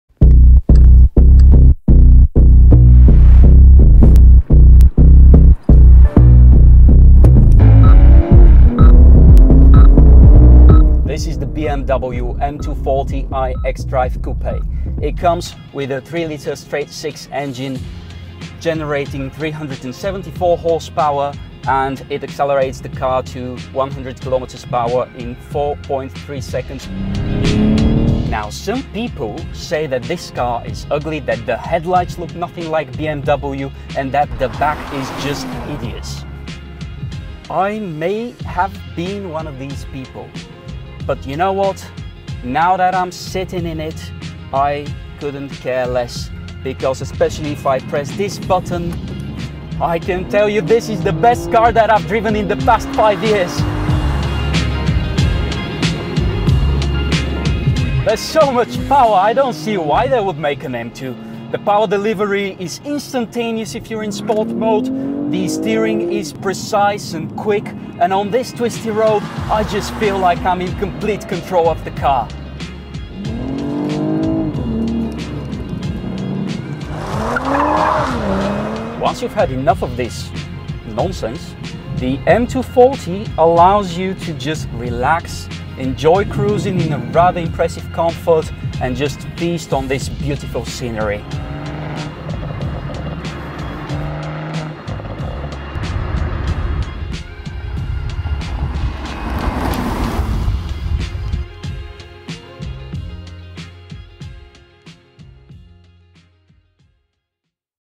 2022 BMW M240i xDrive Coupe - Quick Drive
A short review of the BMW M240 - driving on a twisty road in the mountains, listening to the exhaust noise and enjoying the powerful engine.